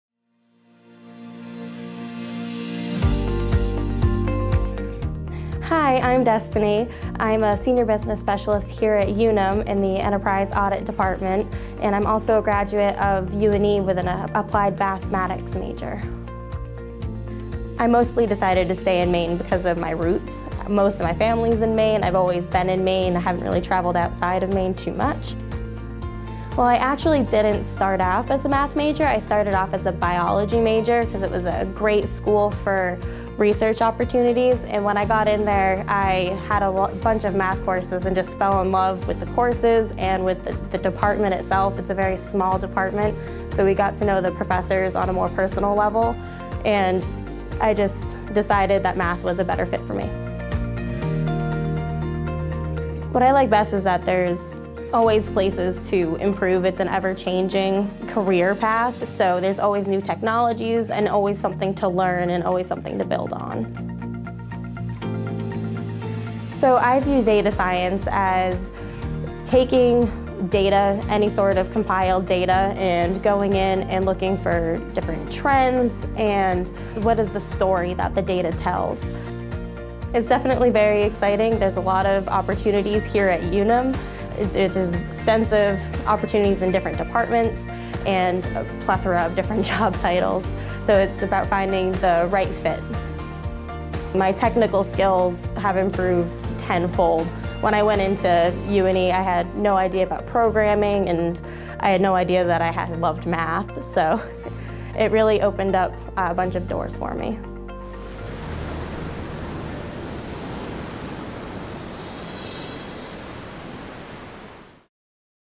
UNE's Data Science Undergraduate Major
UNE’s B.S. in Data Science builds on foundational skills in mathematics and computing, incorporating machine learning, predictive modeling and data visualization. The program infuses project-based learning throughout the curriculum to create career pathways after graduation.